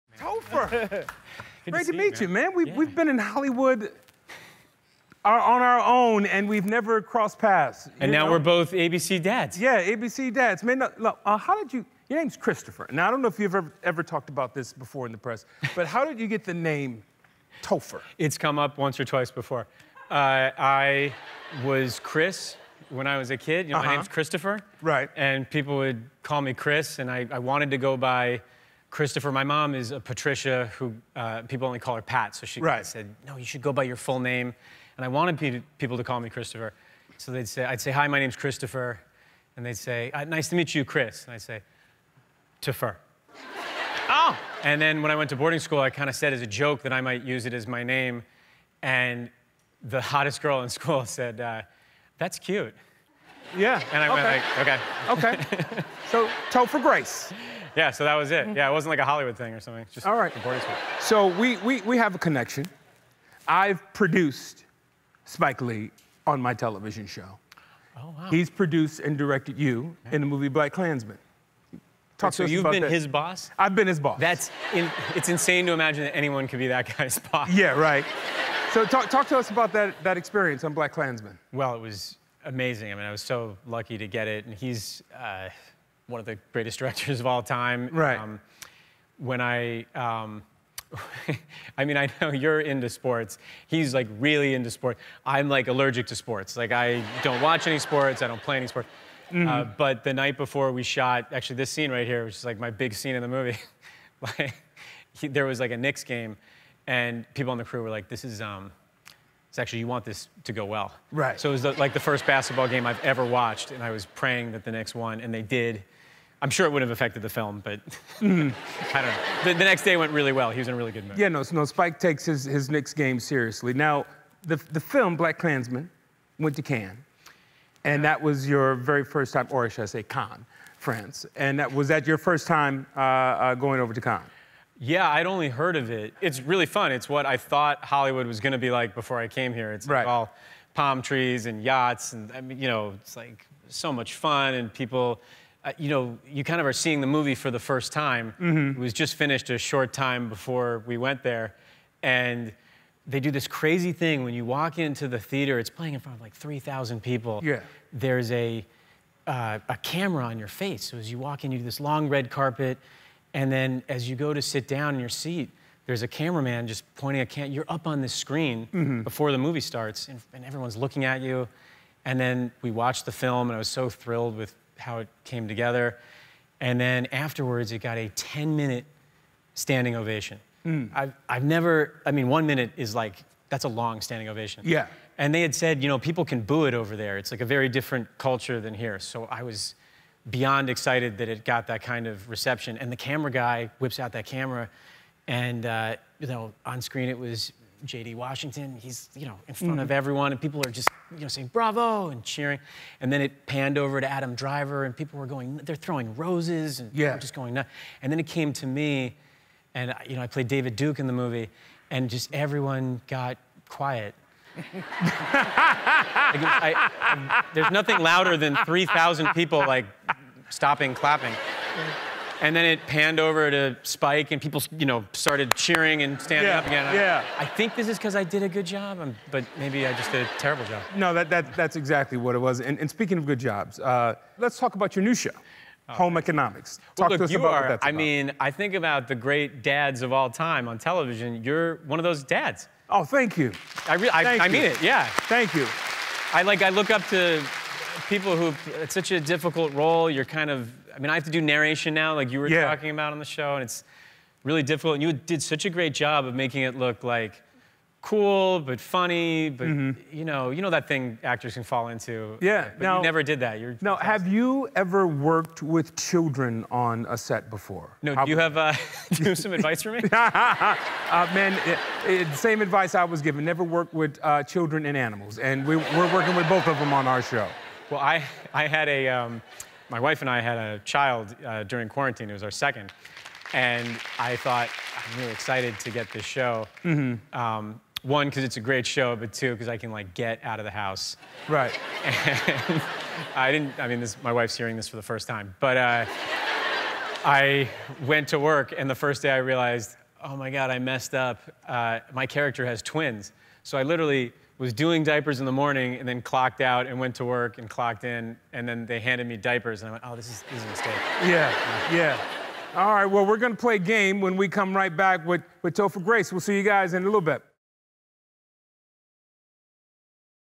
New Dad Topher Grace Realized He Made a Mistake with New TV Show
During quarantine, Topher Grace welcomed his second child, and when he signed up to star in the new show "Home Economics," he was excited to leave the house. He later realized he messed up after forgetting his character is a father of twin babies! The actor also talked about the awkward moment when he was the only cast member not to receive applause after the Cannes Film Festival screening of his acclaimed movie "BlacKkKlansman," and how he got the nickname Topher.